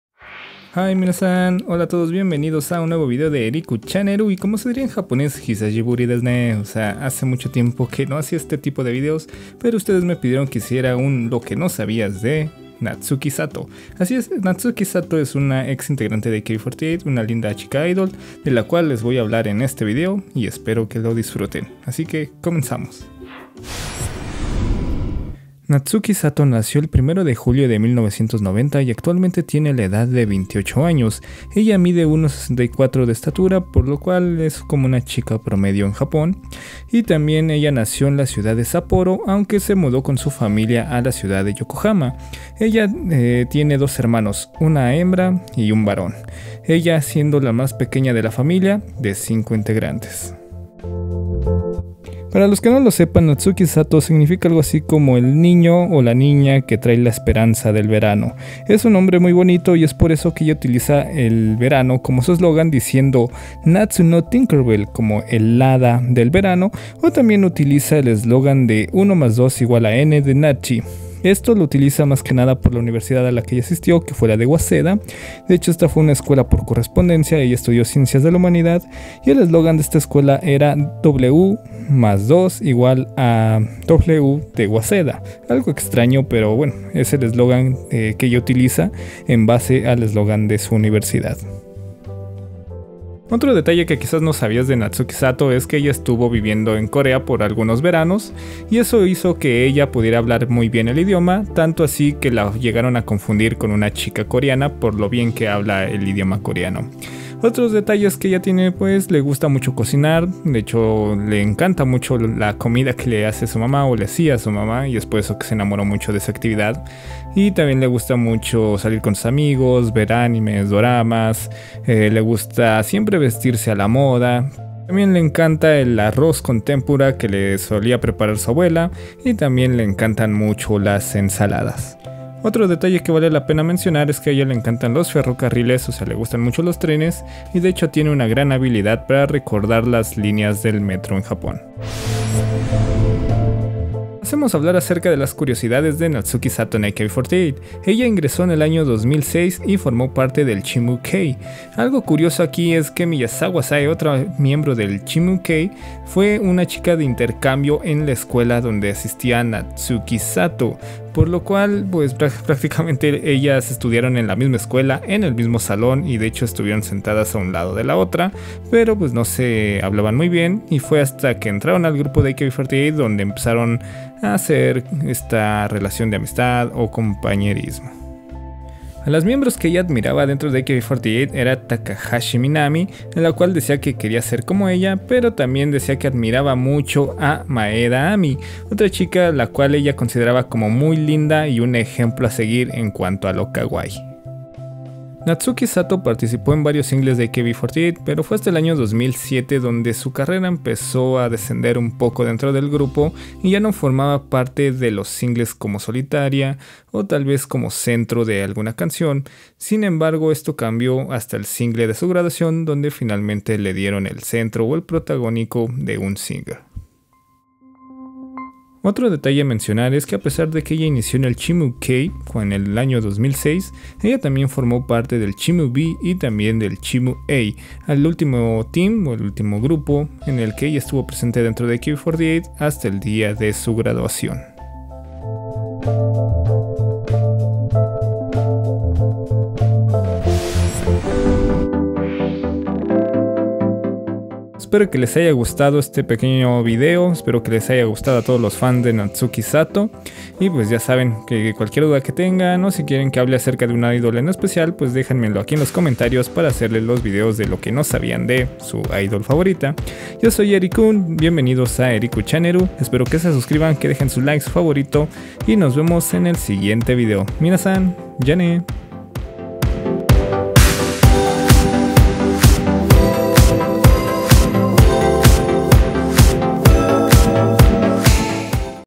Lo que no sabías de: natsuki sato
y a  por su membresia!
Meta de suscriptores: . 17% 17.2K/100K

Finalmente aquí les dejo el video que me habían solicitado hablando acerca de esta hermosa ex idol